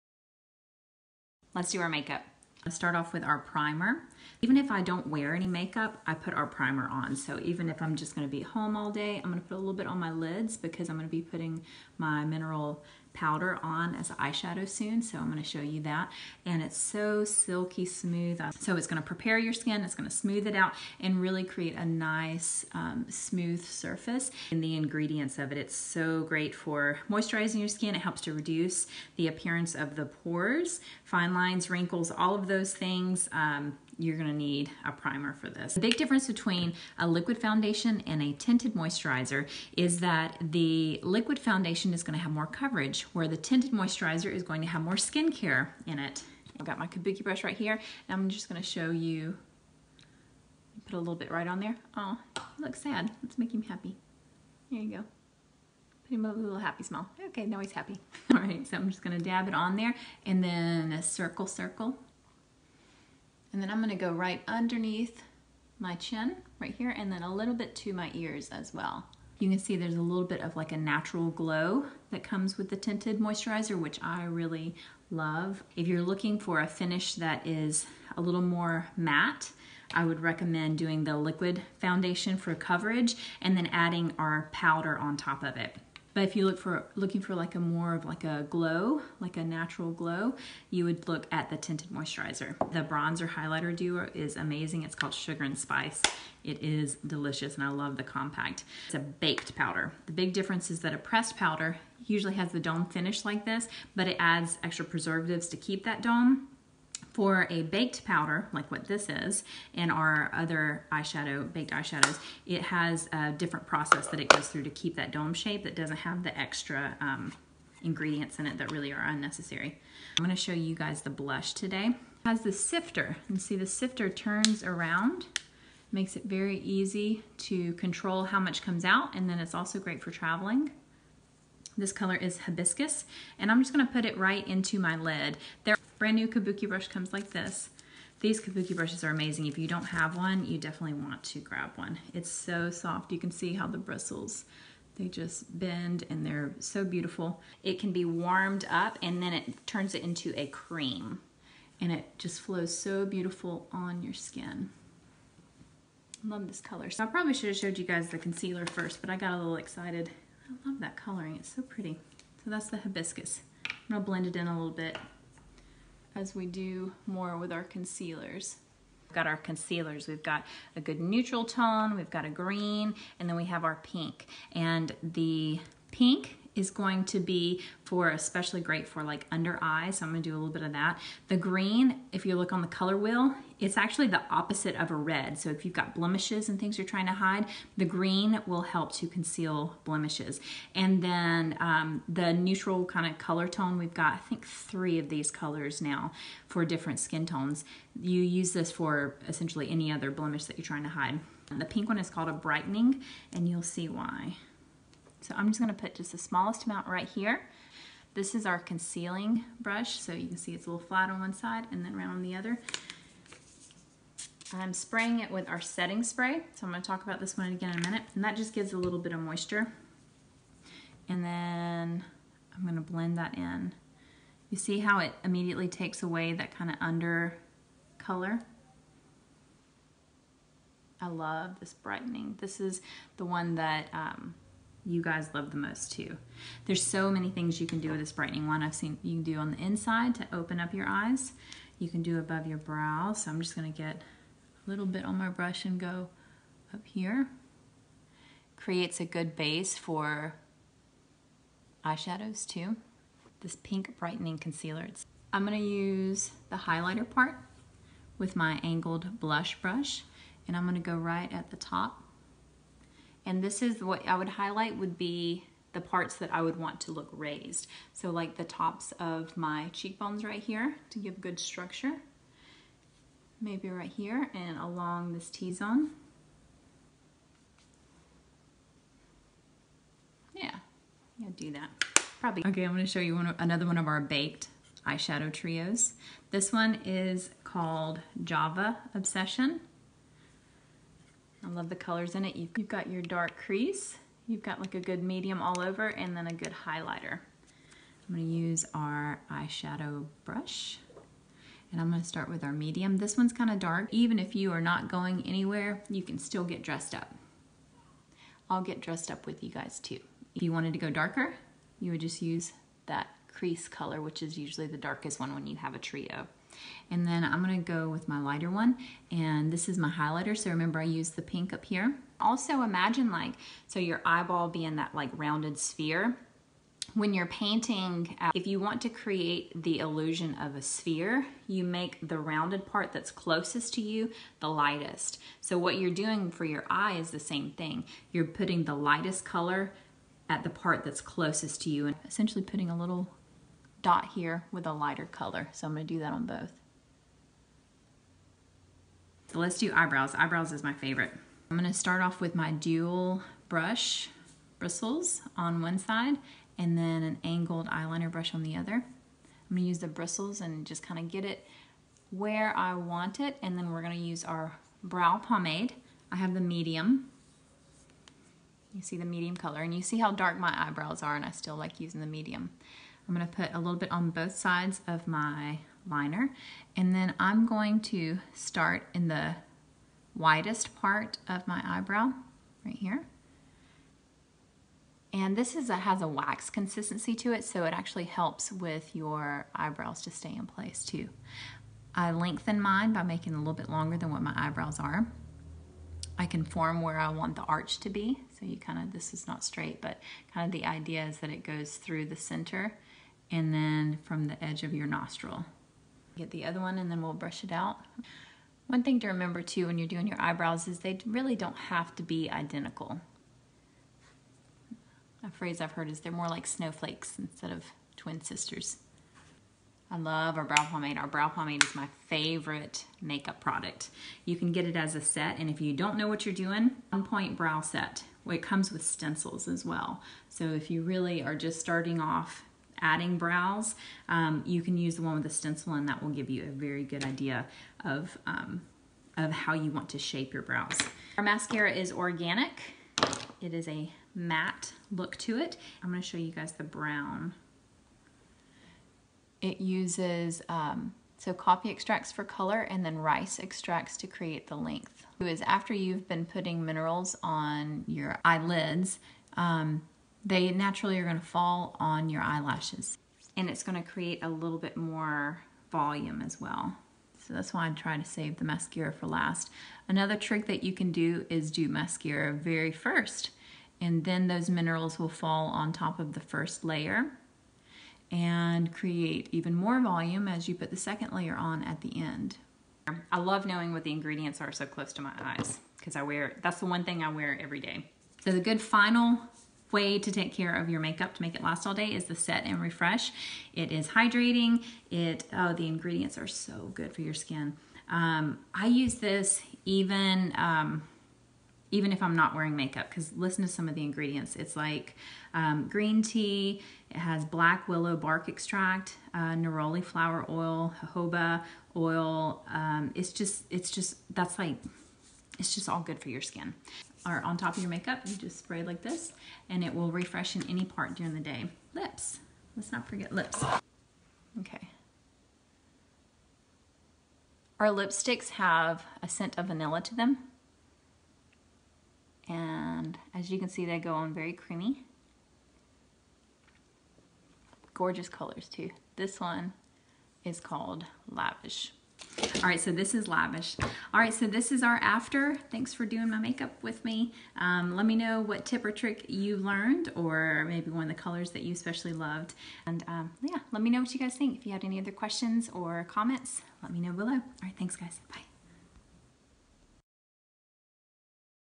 Natural Makeup (tips & tricks) 2021 Edition from Lemongrass Spa
2021 is going to be the year to freshen up your look, switch to clean ingredients and have fun doing it. Let’s start with a clean, natural makeup look from Lemongrass Spa.
**What new tip did you pick up?**